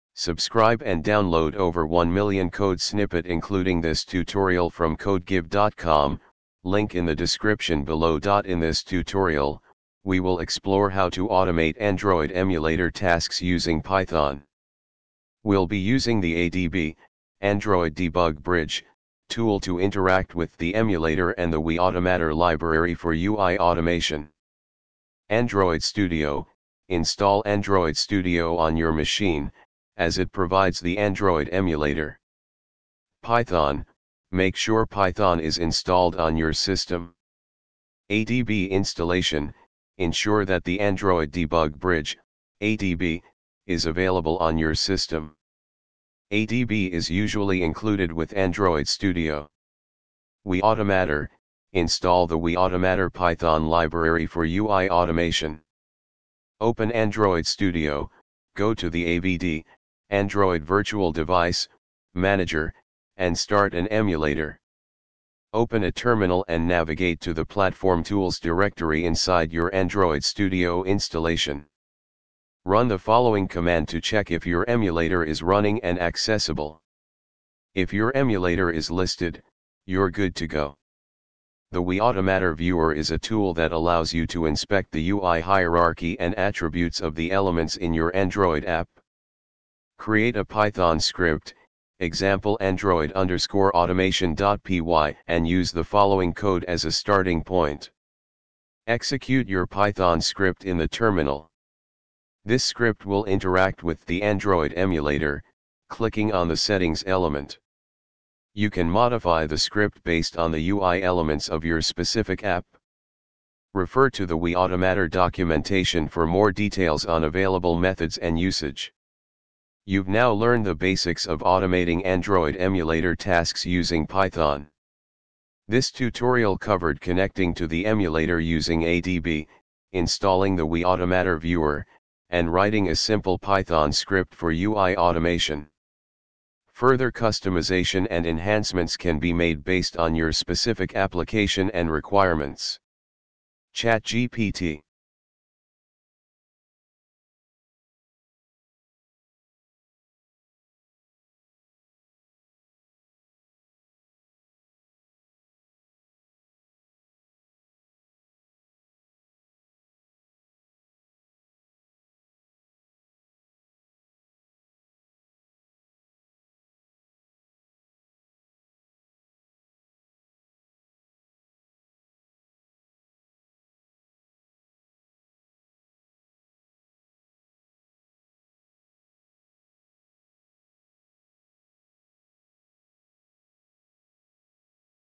python android emulator automation
In this tutorial, we will explore how to automate Android Emulator tasks using Python. We'll be using the adb (Android Debug Bridge) tool to interact with the emulator and the uiautomator library for UI automation.
Android Studio: Install Android Studio on your machine, as it provides the Android Emulator.
Python: Make sure Python is installed on your system.
ADB Installation: Ensure that the Android Debug Bridge (ADB) is available on your system. ADB is usually included with Android Studio.
Uiautomator: Install the uiautomator Python library for UI automation.
Open Android Studio, go to the AVD (Android Virtual Device) Manager, and start an emulator.
Open a terminal and navigate to the "platform-tools" directory inside your Android Studio installation. Run the following command to check if your emulator is running and accessible:
If your emulator is listed, you're good to go.
The Uiautomator Viewer is a tool that allows you to inspect the UI hierarchy and attributes of the elements in your Android app.
Create a Python script (e.g., android_automation.py) and use the following code as a starting point:
Execute your Python script in the terminal:
This script will interact with the Android Emulator, clicking on the "Settings" element. You can modify the script based on the UI elements of your specific app.
Refer to the Uiautomator Documentation for more details on available methods and usage.
You've now learned the basics of automating Android Emulator tasks using Python. This tutorial covered connecting to the emulator using ADB, installing the Uiautomator Viewer, and writing a simple Python script for UI automation. Further customization and enhancements can be made based on your specific application and requirements.
ChatGPT